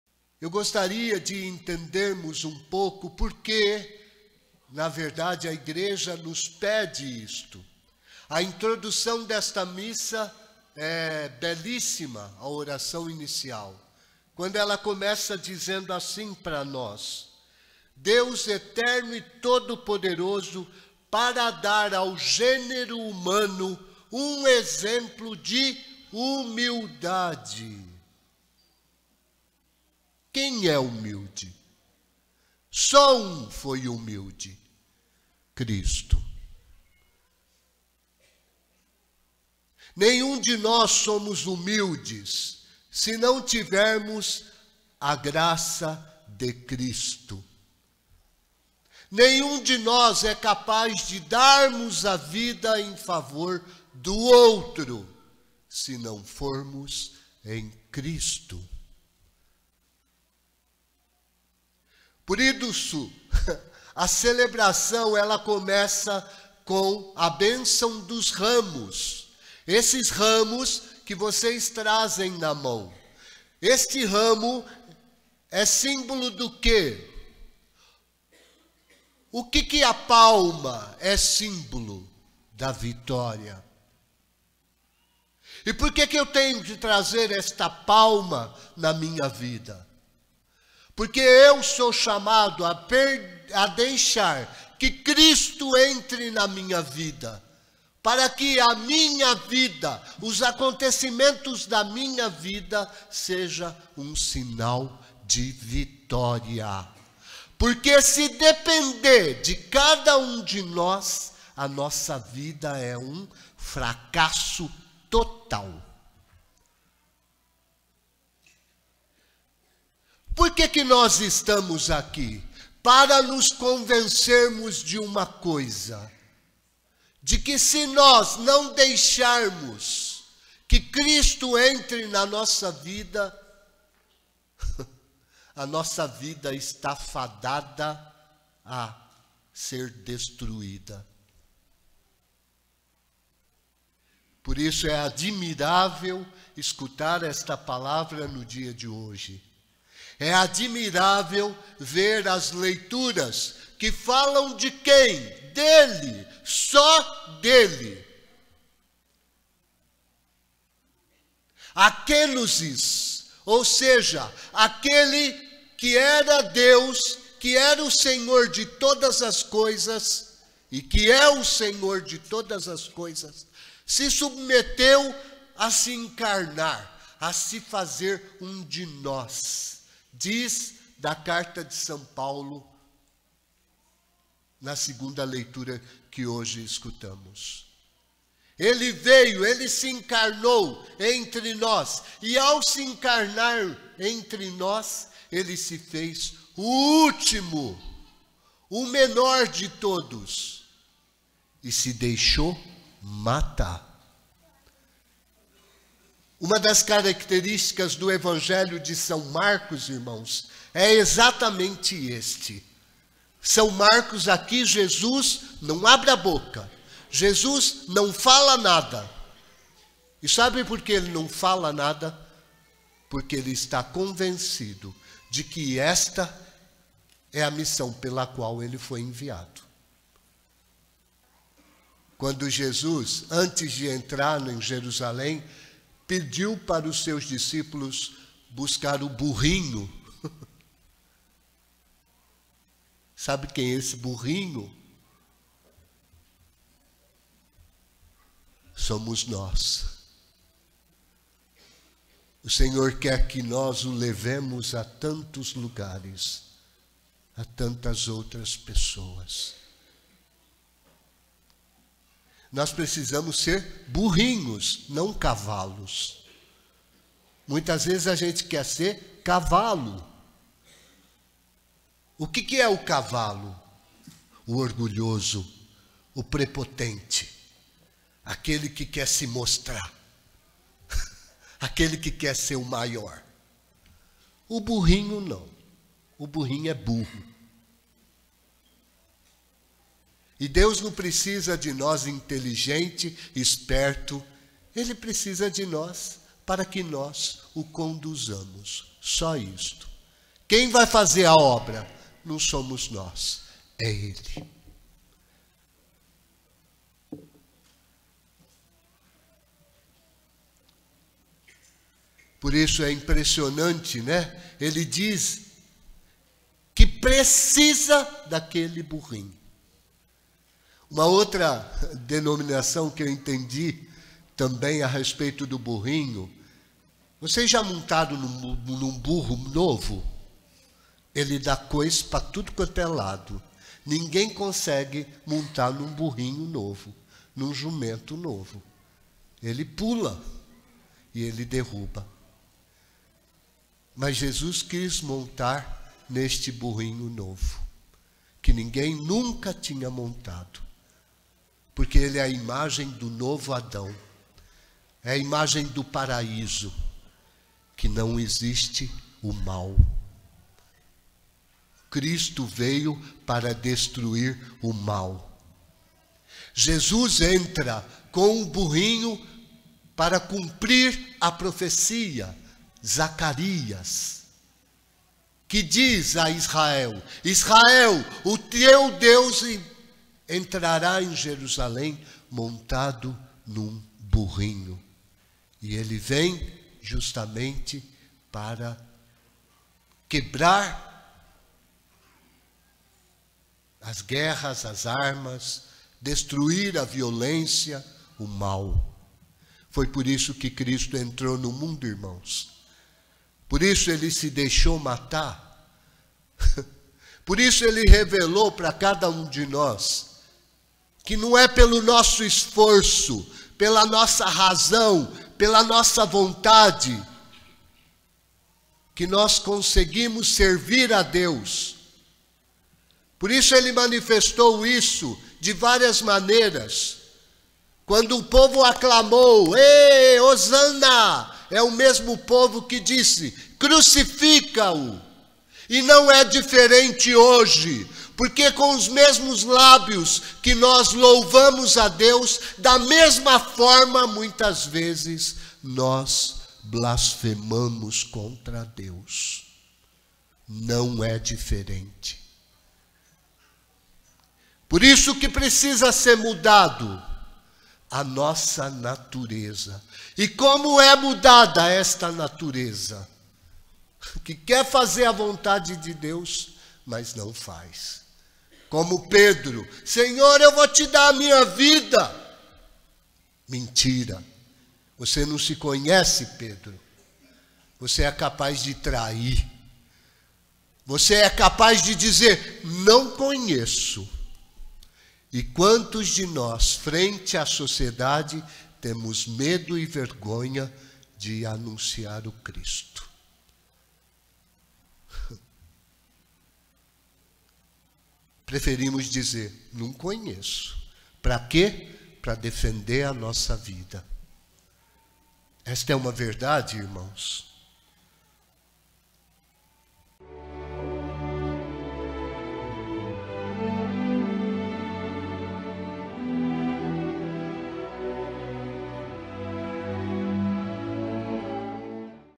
Cristo, exemplo de humildade
Jesus entra com o burrinho para cumprir a profecia

Padre Omar Rachad Ghinaim

DEIXE O SEU LIKE  👍🏼 / COMENTE